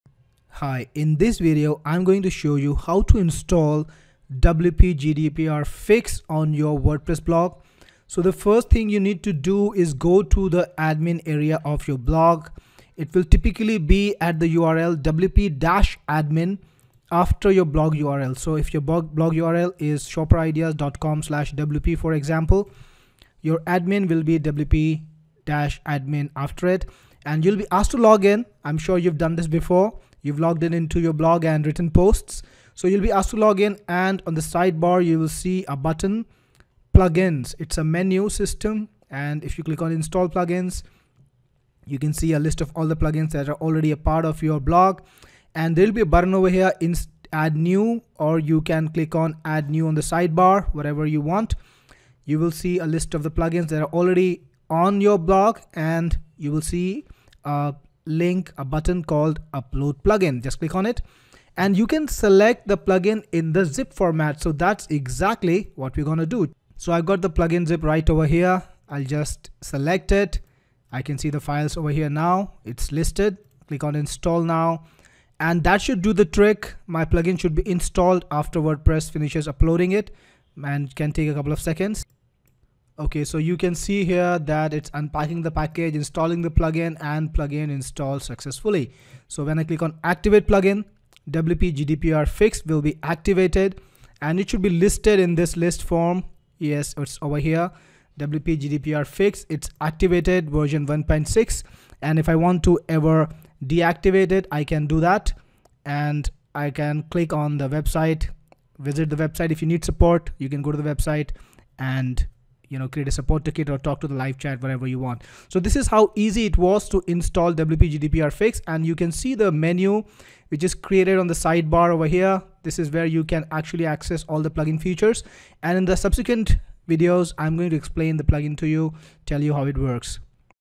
Installing WP GDPR Fix
Install the WP GDPR plugin seamlessly on your Wordpress blog with this 2 minute tutorial.

It's a very simple process, follow the instructions and you will be done in no time. In case you may still have doubts or face challenges, our support is available 24 hours.

Once the plugin is installed we will take you through all its features in the subsequent videos.